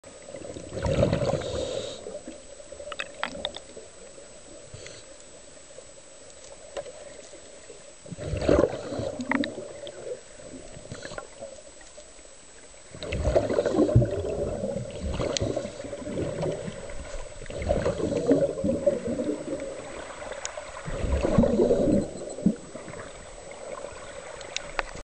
Scuba Diving in Cyprus, Caves
Surfacing through underwater caves!
Caves - Cape Greko, CYPRUS.